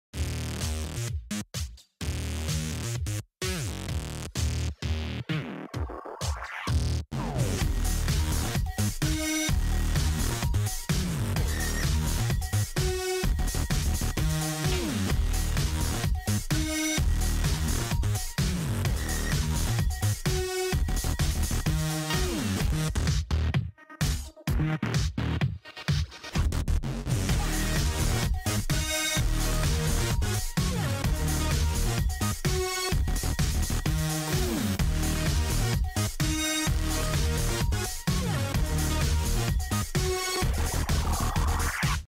The Tower Of Doom a LEGO Star Wars Stop Motion
Today there’s a witch stuck on top of a tower of dooom and some clone try to rescue here!